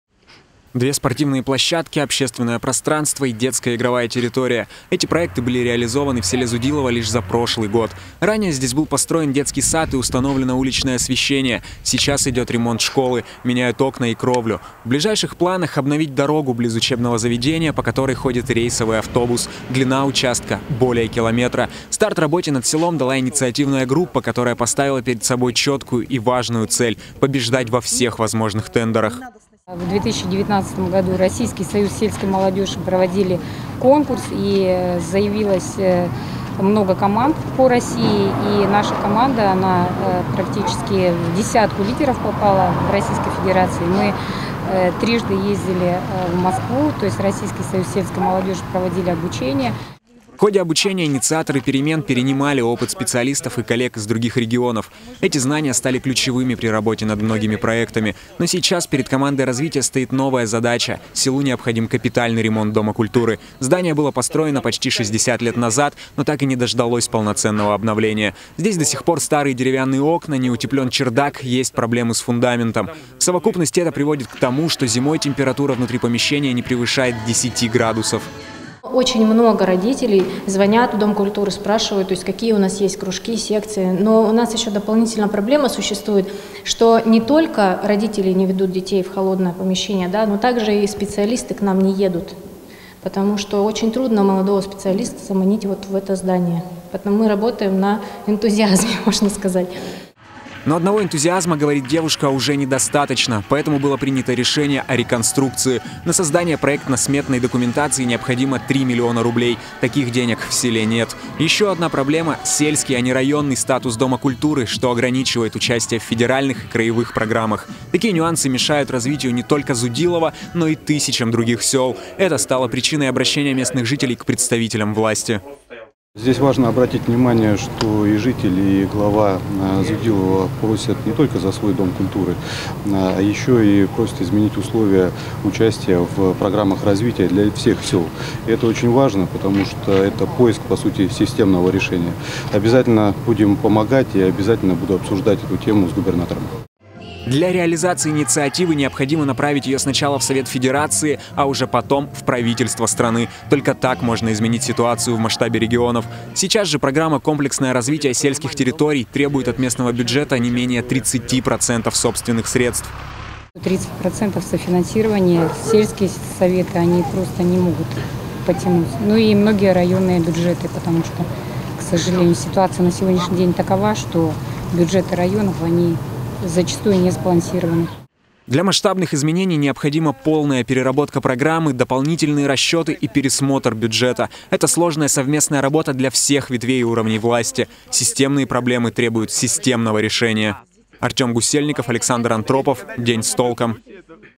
Селу Зудилово не хватает средств для реконструкции единственного Дома культуры
В противовес мифам об умирающей русской провинции село Зудилово продолжает развиваться. Ежегодно здесь строят новые объекты инфраструктуры и модернизируют старые.  Однако сейчас село столкнулось с проблемой – для реконструкции единственного Дома культуры не хватает средств, а попасть в госпрограмму практически невозможно